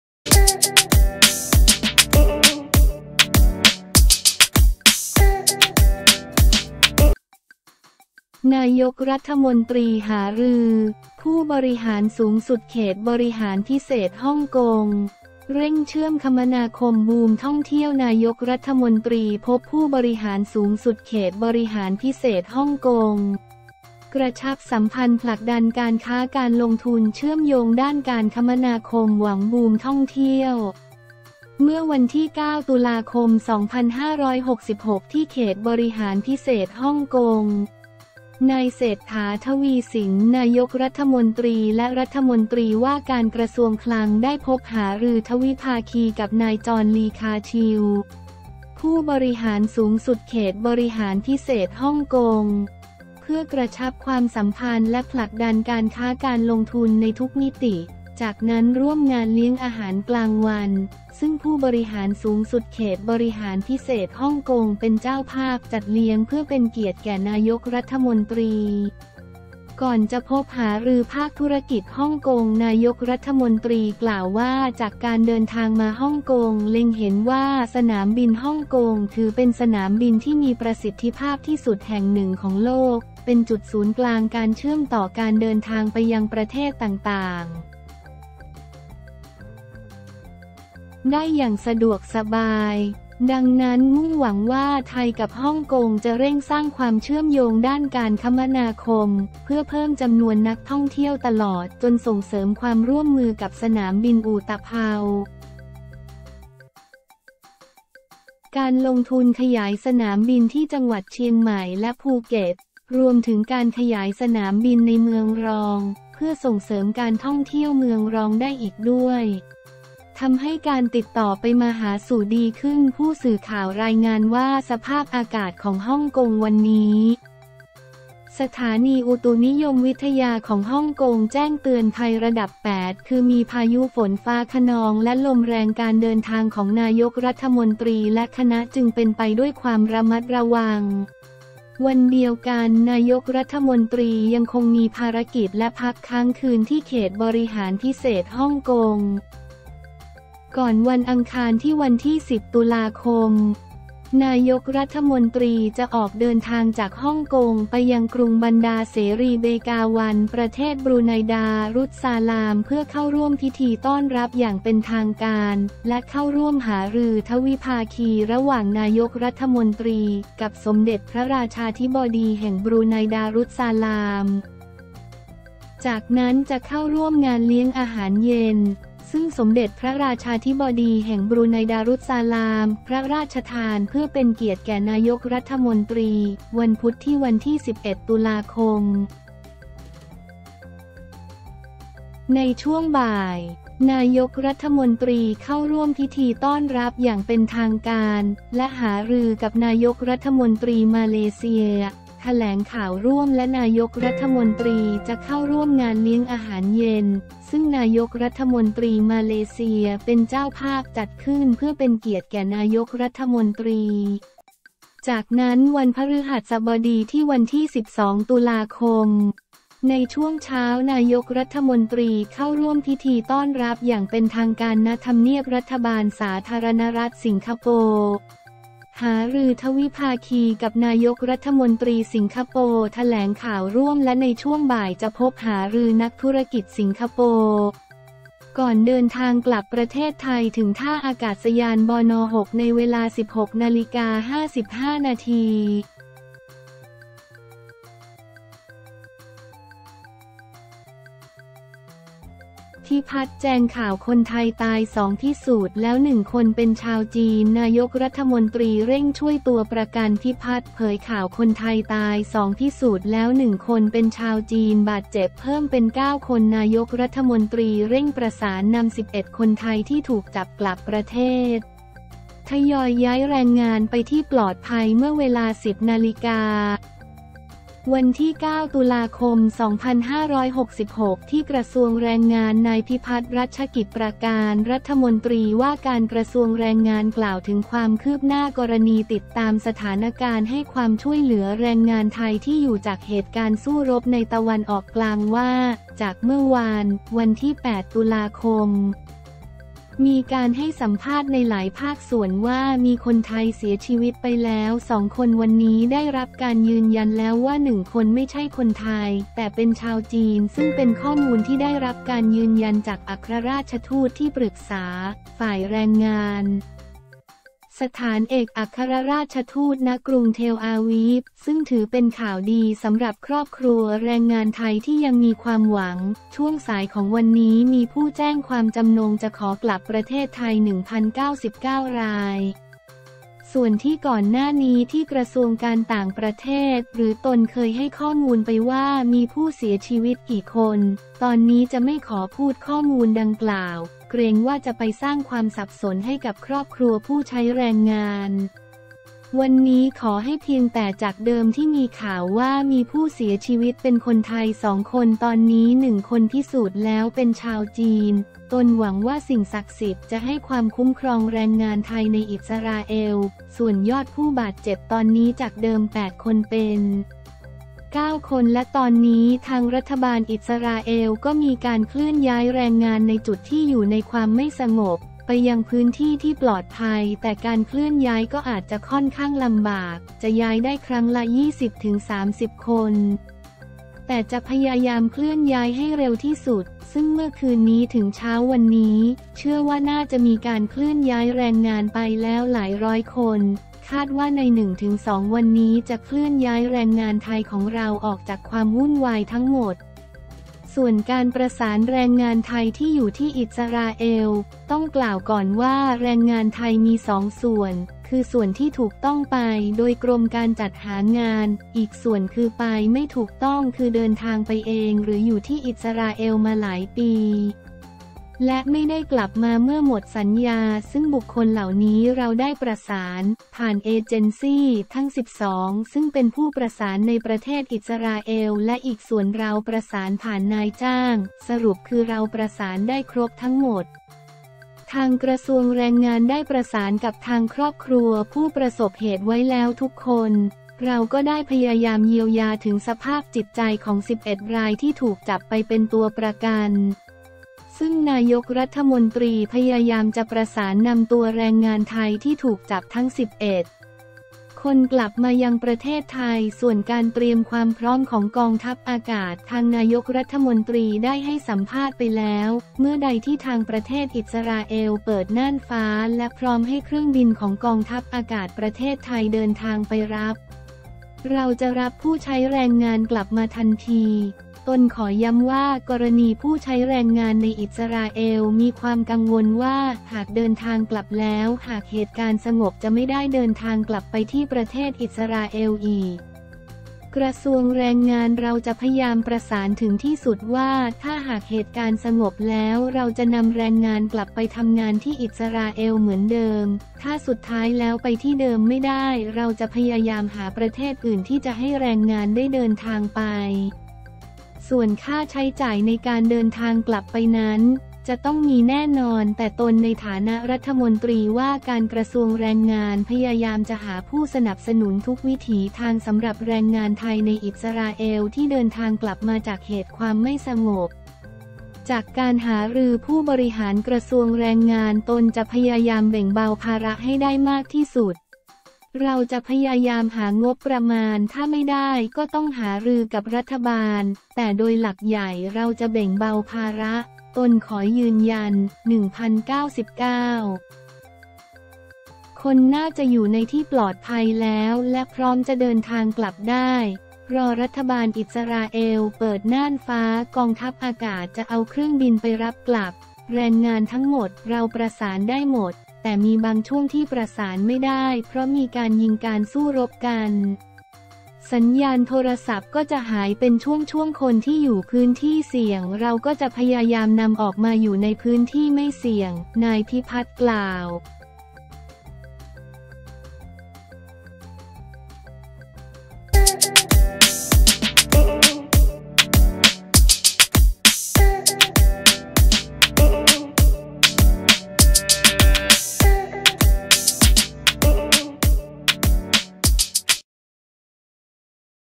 นายกฯ หารือ ผู้บริหารสูงสุดเขตบริหารพิเศษฮ่องกง เร่งเชื่อมคมนาคม บูมท่องเที่ยว
นายกฯ พบผู้บริหารสูงสุดเขตบริหารพิเศษฮ่องกง กระชับสัมพันธ์ ผลักดันการค้าการลงทุน เชื่อมโยงด้านการคมนาคม หวังบูมท่องเที่ยว
เมื่อวันที่ 9 ต.ค.2566 ที่เขตบริหารพิเศษฮ่องกง นายเศรษฐา ทวีสิน นายกรัฐมนตรี และรมว.คลัง ได้พบหารือทวิภาคีกับนายจอห์น ลี คา-ชิว ผู้บริหารสูงสุดเขตบริหารพิเศษฮ่องกง เพื่อกระชับความสัมพันธ์และผลักดันการค้าการลงทุนในทุกมิติ จากนั้นร่วมงานเลี้ยงอาหารกลางวัน ซึ่งผู้บริหารสูงสุดเขตบริหารพิเศษฮ่องกงเป็นเจ้าภาพจัดเลี้ยงเพื่อเป็นเกียรติแก่นายกรัฐมนตรี ก่อนจะพบหารือภาคธุรกิจฮ่องกง
นายกฯ กล่าวว่า จากการเดินทางมาฮ่องกง เล็งเห็นว่าสนามบินฮ่องกงถือเป็นสนามบินที่มีประสิทธิภาพที่สุดแห่งหนึ่งของโลก เป็นจุดศูนย์กลางการเชื่อมต่อการเดินทางไปยังประเทศต่างๆ ได้อย่างสะดวกสบาย ดังนั้น มุ่งหวังว่าไทยกับฮ่องกงจะเร่งสร้างความเชื่อมโยงด้านการคมนาคม เพื่อเพิ่มจำนวนนักท่องเที่ยว ตลอดจนส่งเสริมความร่วมมือกับสนามบินอู่ตะเภา การลงทุนขยายสนามบินที่จังหวัดเชียงใหม่และภูเก็ต รวมถึงการขยายสนามบินในเมืองรอง เพื่อส่งเสริมการท่องเที่ยวเมืองรองได้อีกด้วย ทำให้การติดต่อไปมาหาสู่ดีขึ้น
ผู้สื่อข่าวรายงานว่า สภาพอากาศของฮ่องกงวันนี้ สถานีอุตุนิยมวิทยาของฮ่องกงแจ้งเตือนภัยระดับ 8 คือมีพายุฝนฟ้าคะนองและลมแรง การเดินทางของนายกฯและคณะ จึงเป็นไปด้วยความระมัดระวัง

วันเดียวกัน นายกฯ ยังคงมีภารกิจและพักค้างคืนที่เขตบริหารพิเศษฮ่องกง ก่อนวันอังคารที่ 10 ต.ค. นายกฯ จะออกเดินทางจากฮ่องกง ไปยังกรุงบันดาร์เสรีเบกาวัน ประเทศบรูไนดารุสซาลาม เพื่อเข้าร่วมพิธีต้อนรับอย่างเป็นทางการ และเข้าร่วมหารือทวิภาคีระหว่างนายกฯ กับสมเด็จพระราชาธิบดีแห่งบรูไนดารุสซาลาม จากนั้นจะเข้าร่วมงานเลี้ยงอาหารเย็น ซึ่งสมเด็จพระราชาธิบดีแห่งบรูไนดารุสซาลามพระราชทานเพื่อเป็นเกียรติแก่นายกฯ
วันพุธที่ 11 ต.ค. ในช่วงบ่าย นายกฯเข้าร่วมพิธีต้อนรับอย่างเป็นทางการ และหารือกับนายกรัฐมนตรีมาเลเซีย แถลงข่าวร่วม และนายกรัฐมนตรีจะเข้าร่วมงานเลี้ยงอาหารเย็น ซึ่งนายกฯ มาเลเซียเป็นเจ้าภาพจัดขึ้นเพื่อเป็นเกียรติแก่นายกฯ

จากนั้นวันพฤหัสบดีที่ 12 ต.ค. ในช่วงเช้า นายกฯ เข้าร่วมพิธีต้อนรับอย่างเป็นทางการ ณ ทำเนียบรัฐบาลสาธารณรัฐสิงคโปร์ หารือทวิภาคีกับนายกฯ สิงคโปร์ แถลงข่าวร่วม และในช่วงบ่ายจะพบหารือนักธุรกิจสิงคโปร์ ก่อนเดินทางกลับประเทศไทยถึงท่าอากาศยาน บน.6 ในเวลา 16.55 น.